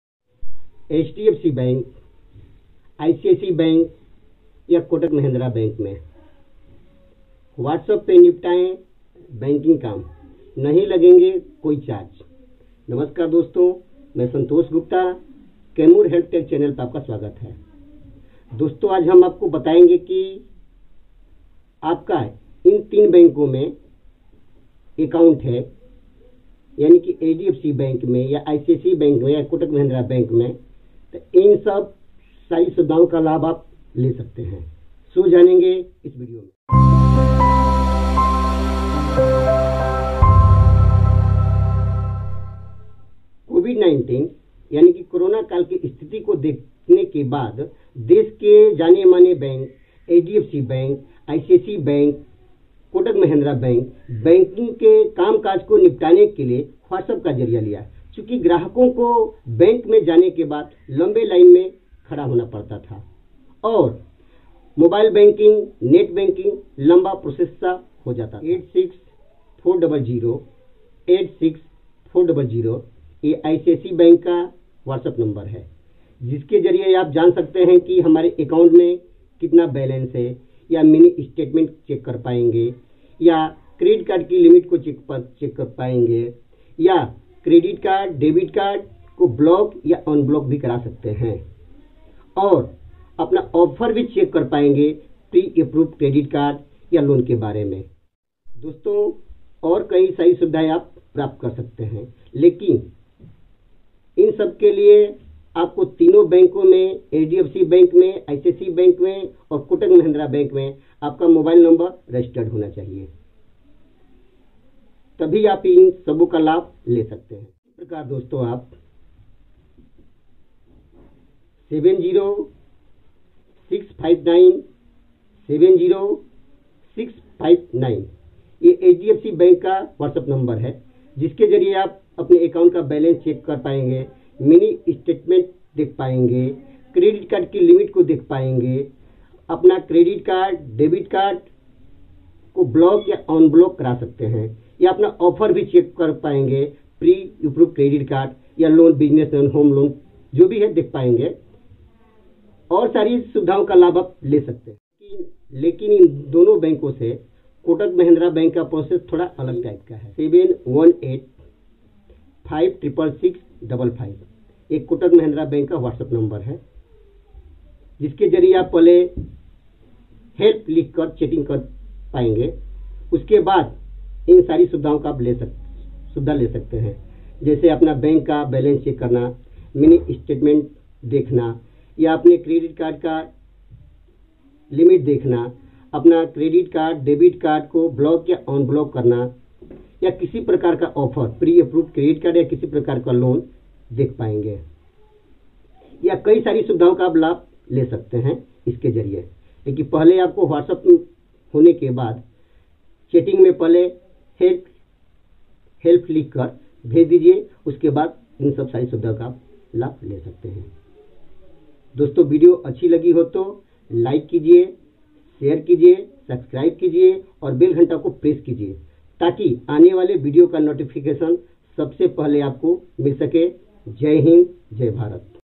HDFC WhatsApp Banking | WhatsApp Banking kaise kare | icici WhatsApp Banking | full guide in Hindi
HDFC WhatsApp Banking , WhatsApp banking kaise kare, ICICI banking WhatsApp, WhatsApp per niptaye Sare kam namaskar doston aaj ham is video mein batayenge ki whatsapp ke jariye aap Bank ka sara kaam kaaj kar sakte hain net banking apna account ka balance check karna mini statement credit card ka limit credit card cash limit aap apna ofer abhi check kar sakte hain Jaise pre approved credit card loan.. home loan personal loan business loan aur bahut sari kam kar sakte hain is whatsapp ke jariye. abhi teen bankon ne ICICI Bank HDFC Bank kotak Mahindra Bank whatsapp ke jarie Apne customer ko Bank aasan kam kar kaj shuru Kiya hai ICICI Bank WhatsApp number HDFC WhatsApp number kotak Mahindra Bank WhatsApp number.. . ine sabhi ke jariye aap Bank ka sara kaam kaaj kar sakte hain email. . follow Instagram. my channel... jay hind jay Bharat